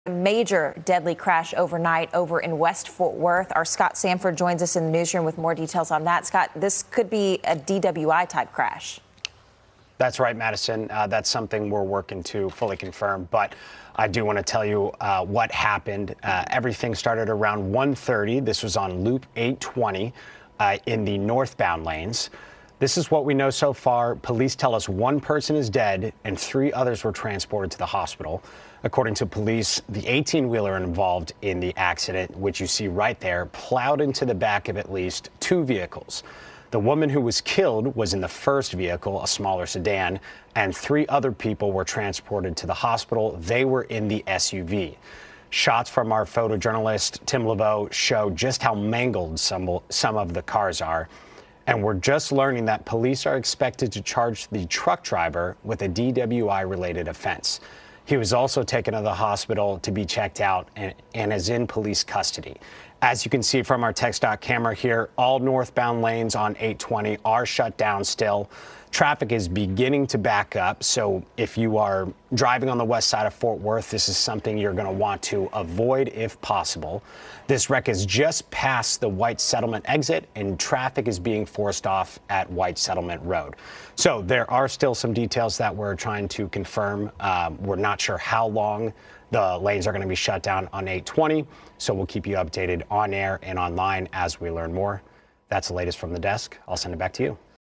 Deadly crash prompts closure on Loop 820 in Fort Worth
An 18-wheeler was heading northbound on Loop 820 when it plowed into the back of at least two vehicles.